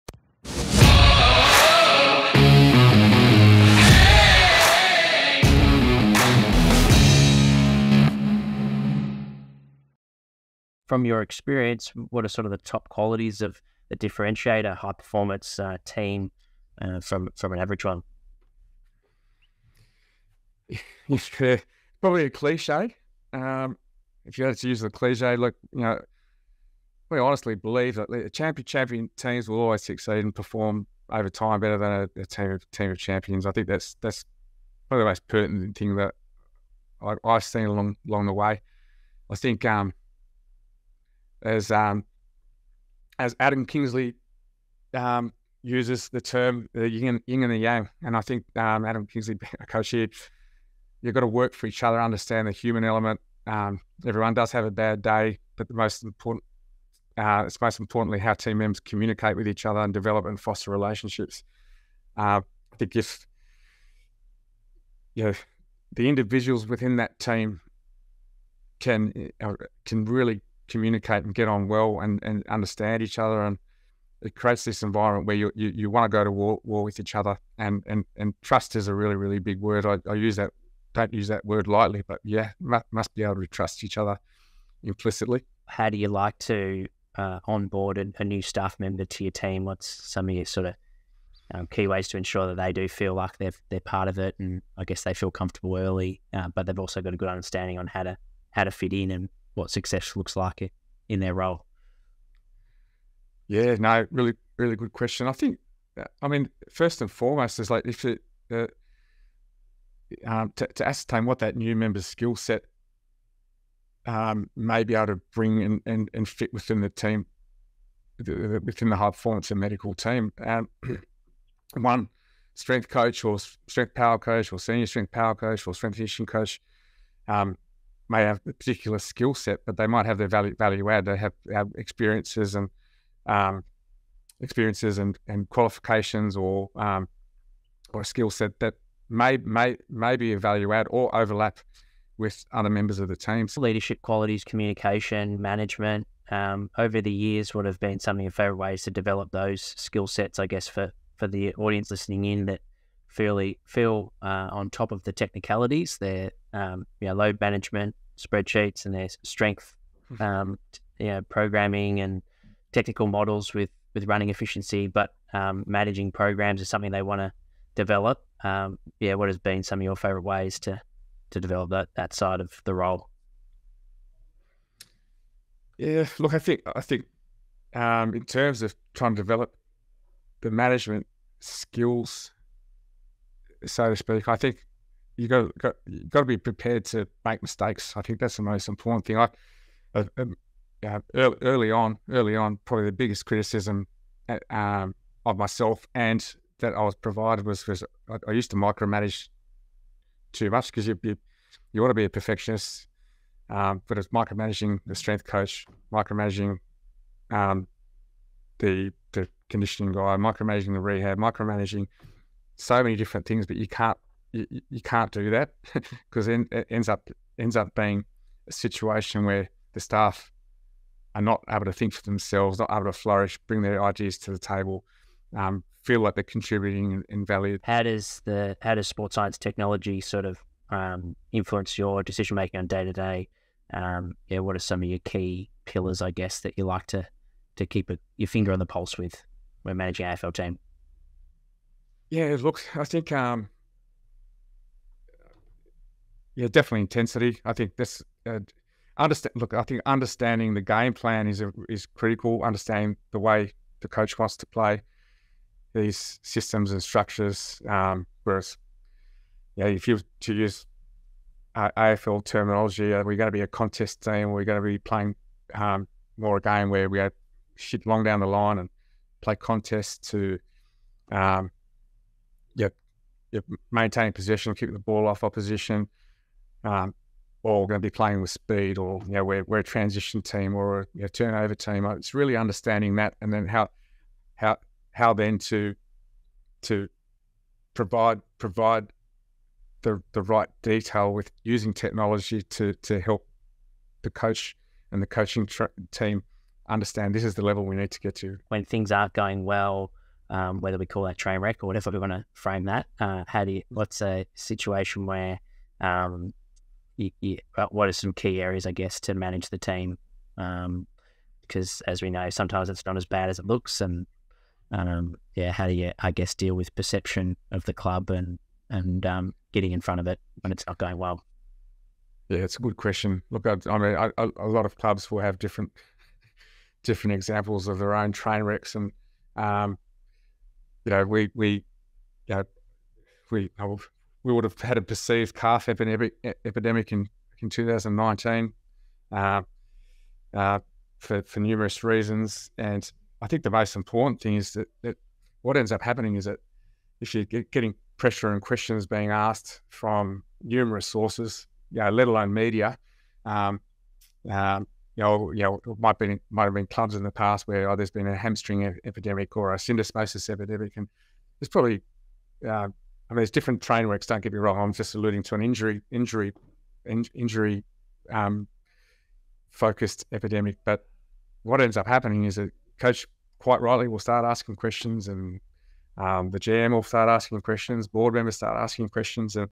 #308 - Nick Poulos, Head of High Performance at GWS Giants Football Club
Nick Poulos is a seasoned Physical Performance Manager with a rich background in pushing the limits of high performance. Join us as Nick shares his journey, influential moments, and the challenges that shaped his career. From the qualities that set high-performance teams apart to groundbreaking training methodologies and leadership development in AFL, Nick offers an unparalleled glimpse behind the scenes of athletic excellence.

Highlights from the episode:

00:00 Traits that distinguish exceptional teams and techniques to bolster mental resilience.
01:25 How to onboard new staff and make sure they fit in the team
02:23 How he develop his leadership qualities
03:47 How sport science technology influence his decision-making and how to leverage it to benefit athletes and coaches
04:58 How to manage 'trainwreck' scenarios and balance rapid decision-making in high-stakes medical situations

Team Cohesion Over Individual Talent:

Nick Poulos emphasizes that a high-performance team benefits more from cohesion and a team-oriented mindset than from a collection of highly talented individuals. The concept of a "team of champions" being less effective than a "champion team" highlights the importance of teamwork and mutual support.

Communication and Trust:

Effective communication and trust are fundamental to the success of high-performance teams. Building and fostering relationships among team members creates an environment of mutual understanding and reliance, which is crucial for overcoming challenges.

Onboarding New Members:

The onboarding process is vital for integrating new staff effectively. Poulos stresses the importance of recognizing new members' unique skills and experiences, helping them fit seamlessly into the team, and understanding their roles and expectations for success.

Development of Management Skills:

Poulos shares insights into developing management skills within a high-performance environment. Learning from mistakes, avoiding micromanagement, and encouraging staff to contribute ideas and take initiative are essential strategies for fostering a productive and innovative team atmosphere.

Use of Technology in Strategy:

The application of sports science and technology plays a significant role in strategic decision-making. Poulos discusses the importance of aligning technology with the coach's game plan and team strategy to ensure optimal performance and understand team dynamics and objectives.

People mentioned:

- Andrew Russell
- Darren Burgess
- Peter Burge
- Mick Newton
- Rob Newton
- Craig Purdam
- Mike Mcgurn
- Andy Friend
- Adam Kingsley
- Dean Bailey
- Phil Walsh
- John Quinn
- Jason Webber
- Greg Haff
- Rob Newton
- Olivia Gerard
- Martin Buchheit
- Adam Kingsley
- Steven Shrig
- Matthew Boyd